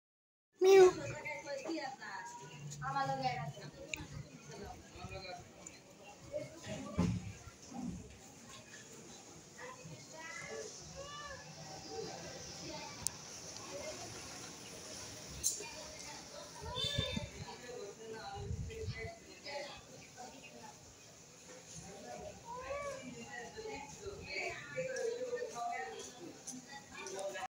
Kitten with it’s mother
I was passing along with the road side.  Suddenly I saw kitten are playing and mother feeding milk one of the kitten. These kitten are very Cute.😻😻